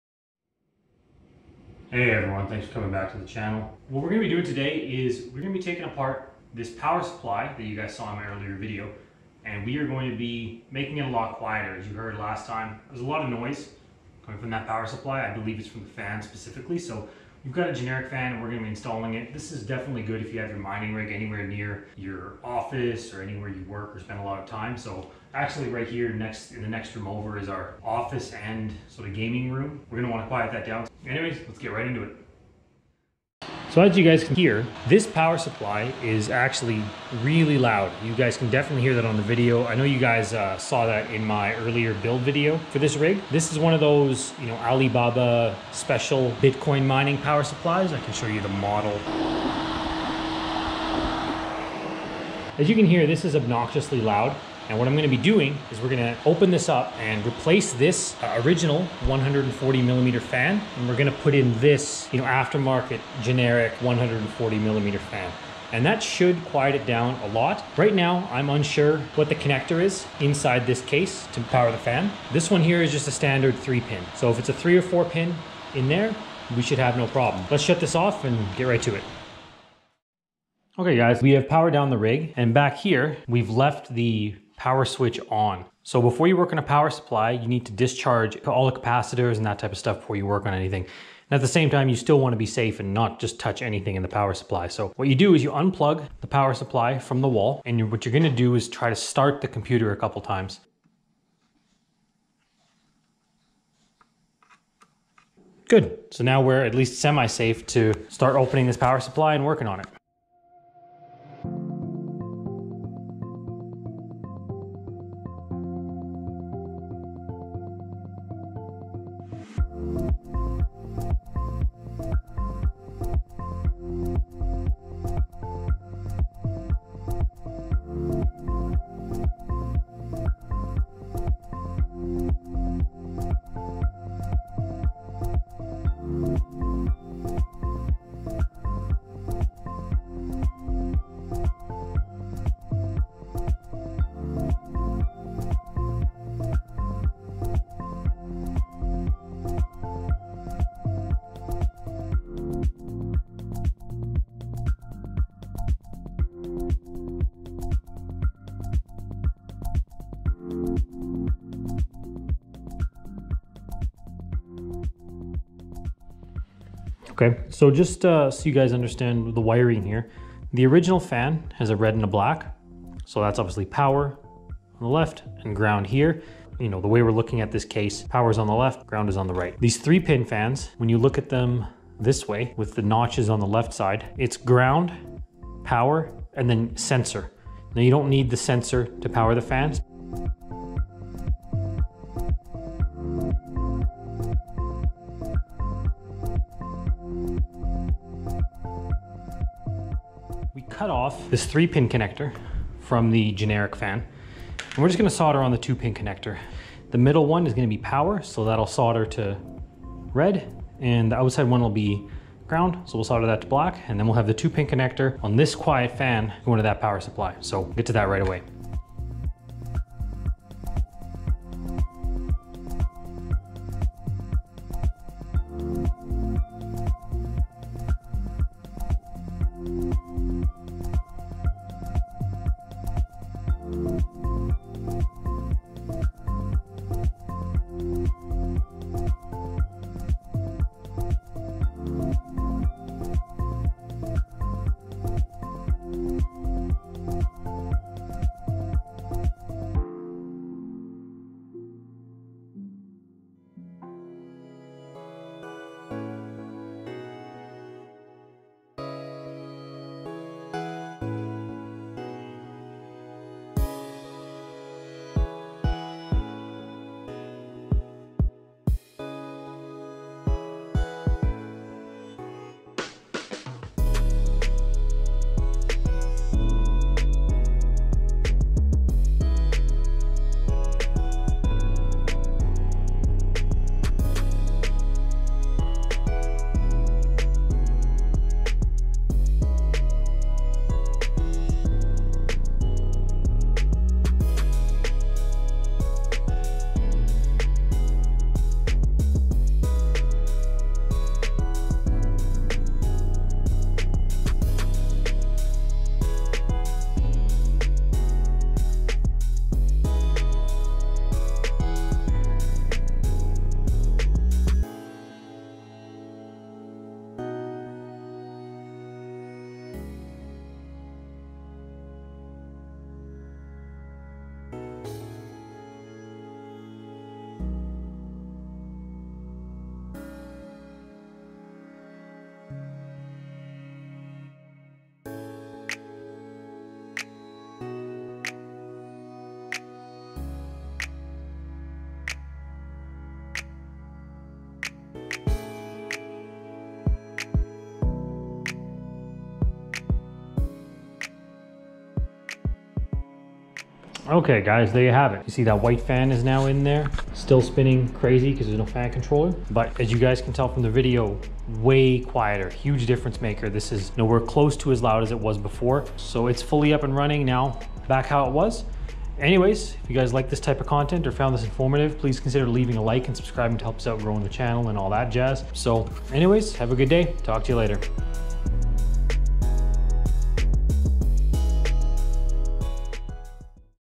Power Supply Too Loud? | Fix Your Mining PSU | Fan Replacement
Guide on how to silence/quiet your Crypto/Bitcoin/Ethereum mining power supplies, especially those cheap ones from China via eBay, Alibaba, and AliExpress.